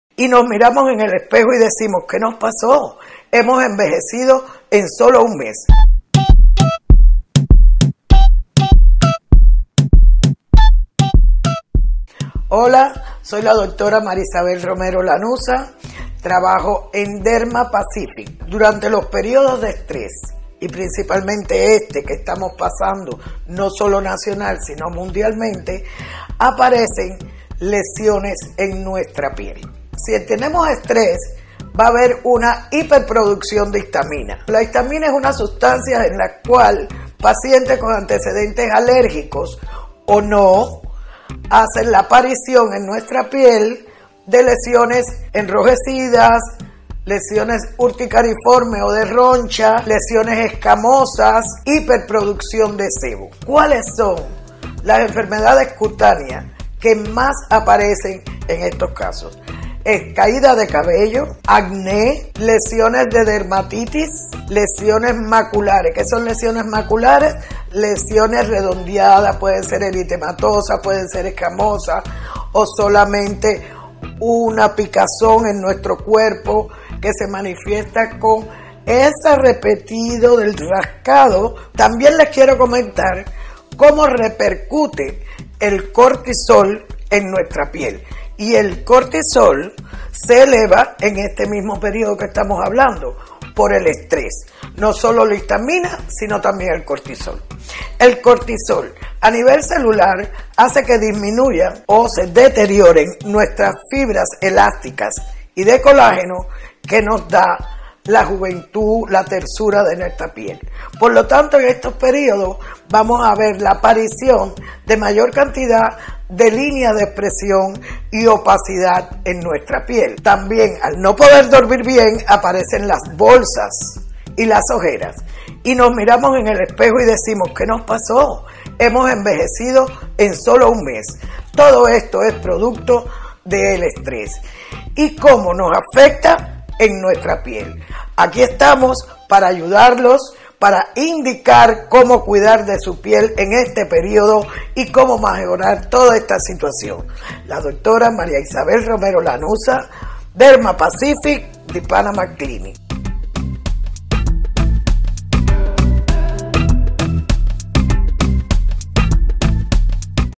El estrés de la pandemia y tu piel
La Dra. María Isabel Romero Lanuza es una de las dermatólogas latinoamericanas con mayor experiencia y conocimiento en le manejo clínico y estético de los problemas del envejecimiento y las afecciones de la piel. En este vídeo te comenta sobre cómo todo el estrés de la pandemia de Covid-19 puede afectar tu piel y las razones por las que encuentras que necesitas ayuda de un dermatólogo especialista con idoneidad. Para mayor información contacta a la Dra.